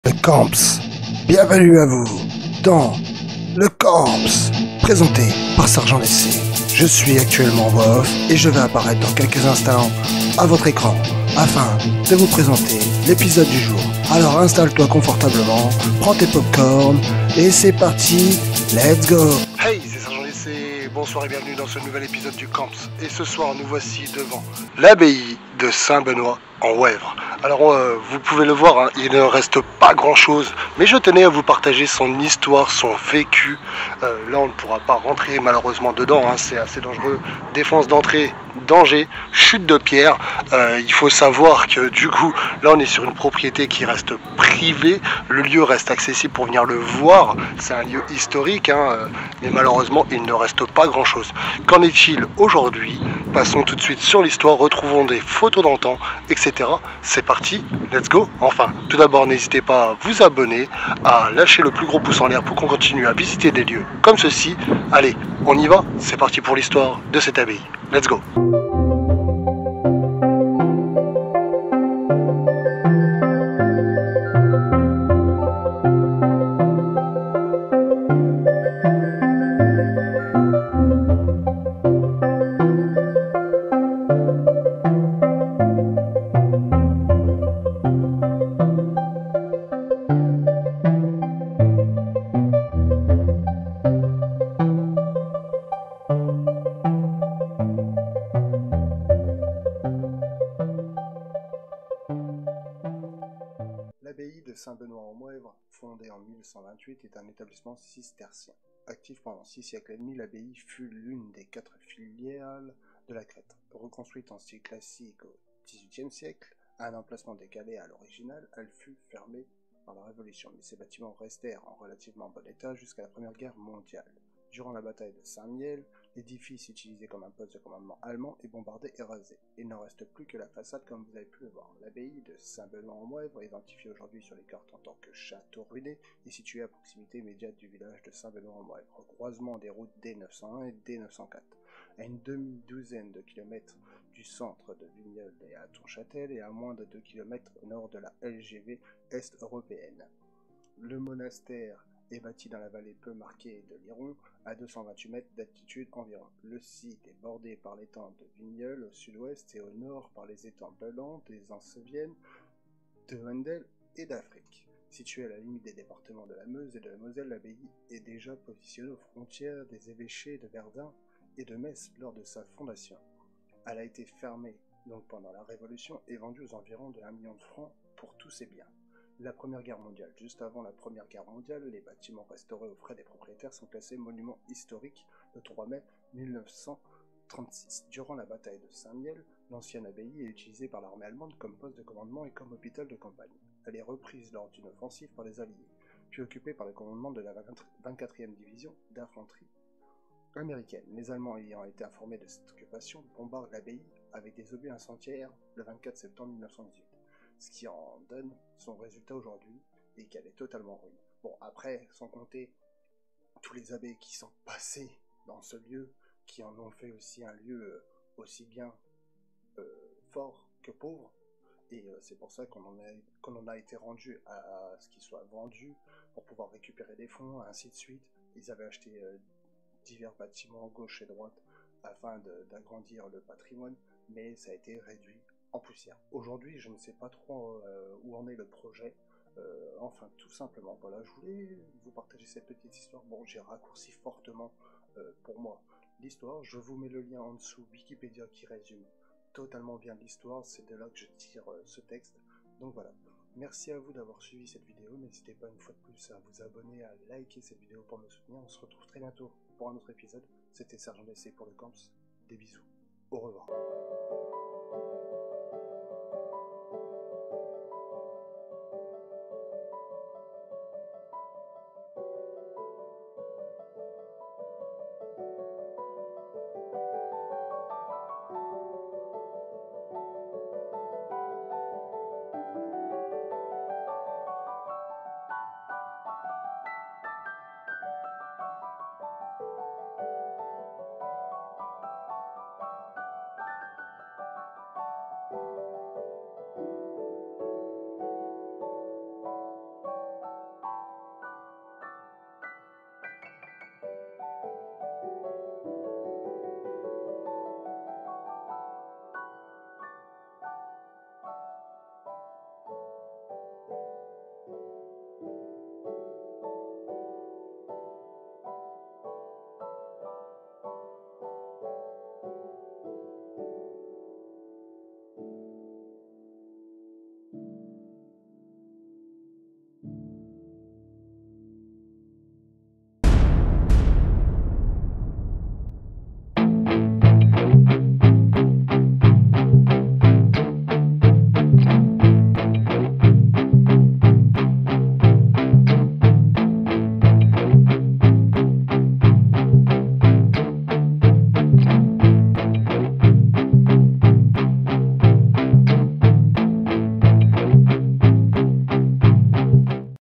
Le Camp's (saison 3) : L' histoire de L' Abbaye de Saint-Benoit-en-Woëvre .
Hey c'est Sergent_DC ,
nouvelle épisode du Camp's ( Saison 3 )
Aujourd'hui , découvrons L'Abbaye de Saint-Benoit-en-Woëvre
lien vers le wiki du lieu :

Bon visionnage , N'hésite pas à LIKER cette vidéo pour me soutenir !